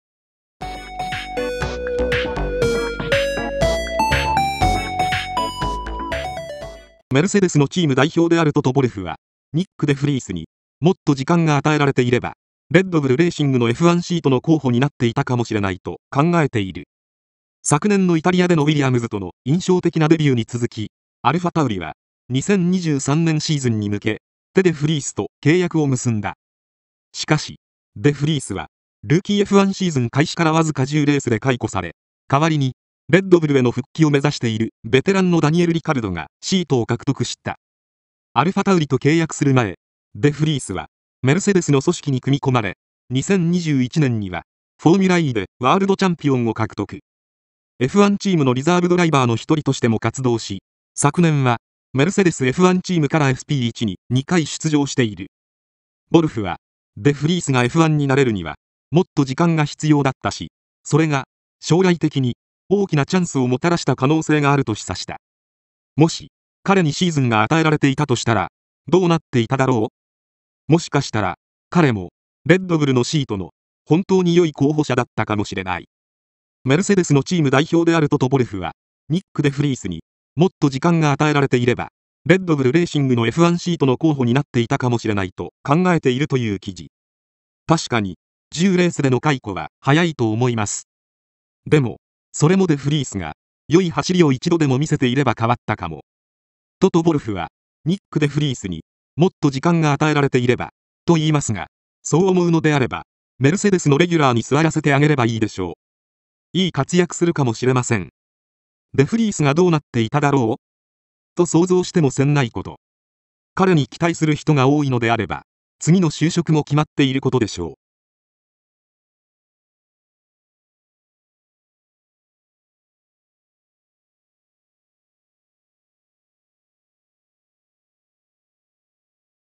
Ｆ1 メルセデスF1代表 「デ・フリースは本来レッドブルの候補に入れる逸材」 ’23 08 11 デ・フリース トト・ヴォルフ
Ｆ1情報　メルセデスF1代表 「デ・フリースは本来レッドブルの候補に入れる逸材」とコメント。
　　　　　何を狙っているんでしょうね。